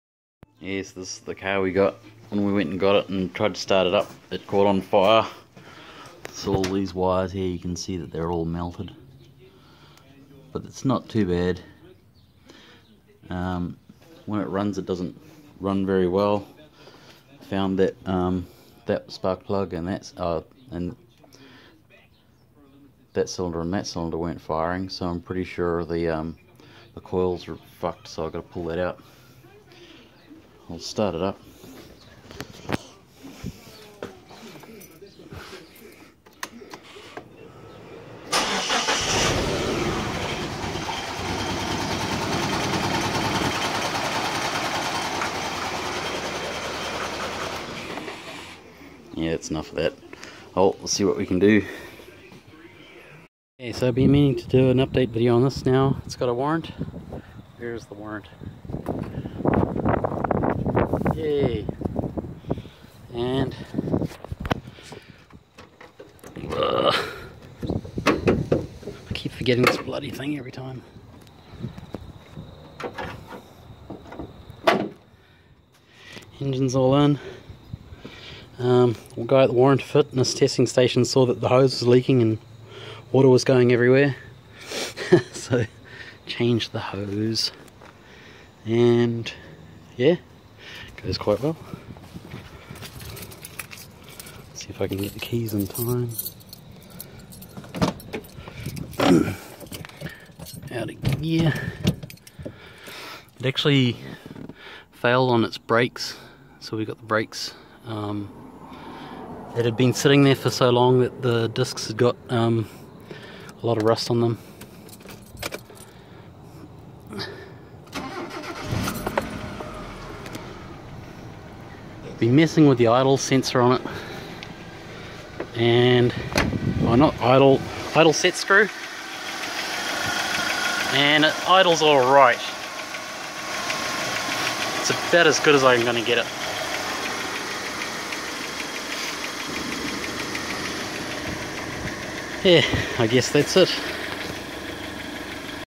Finally all done for the Mitsubishi Galant 4G67
At the start of the video you'll see the donor engine that we got. It caught on fire when we tried to start it upon sale. The return line for the injectors was disconnected. Hahaha, once the fire was out I found that it was only running on 2 cylinders. Changing Coils or igniters made no difference. Pretty sure the fire had nothing to do with it, because it was bouncing around before it caught fire. So I just bit the bullet and conducted the engine swap anyway. Turned out the engine ran fine, didn't over heat or anything strange when installed. I did however take the throttle body of the dead engine because it was set up better for controlling the idle on the new engine.